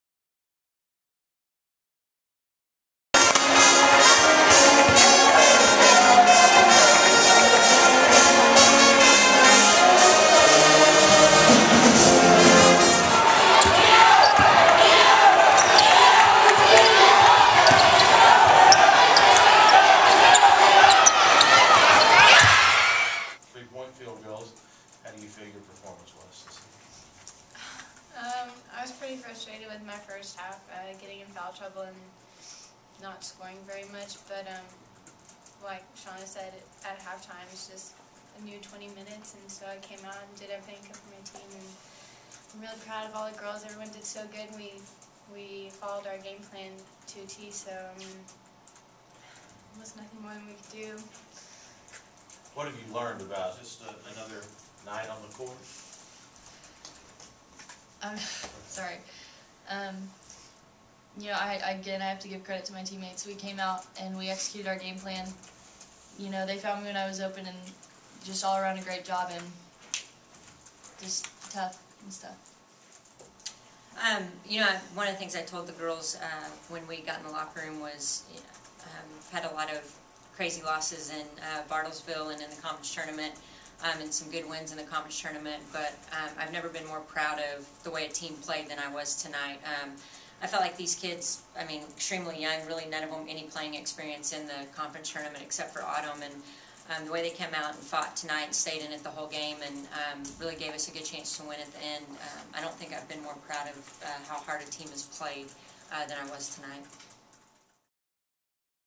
Abilene Christian Press Conference
Northeastern State 65, Abilene Christian 59